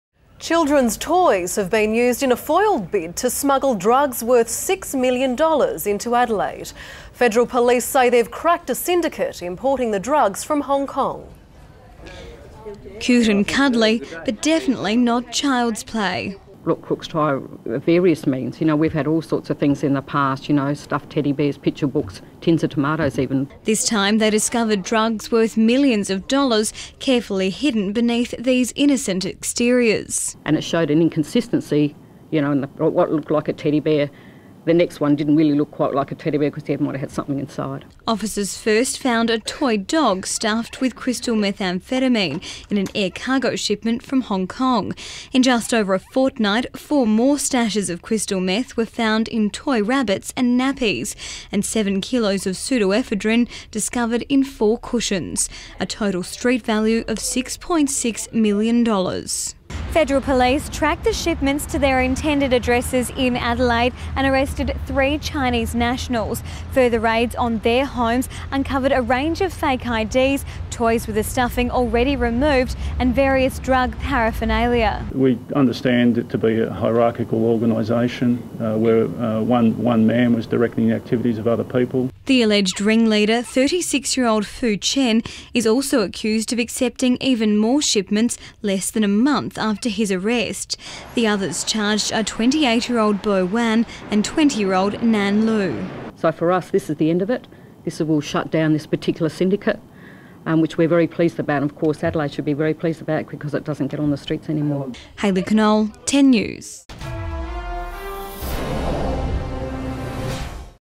Trojan toys
Children's toys have been used in a foiled bid to smuggle drugs worth six million dollars into Adelaide.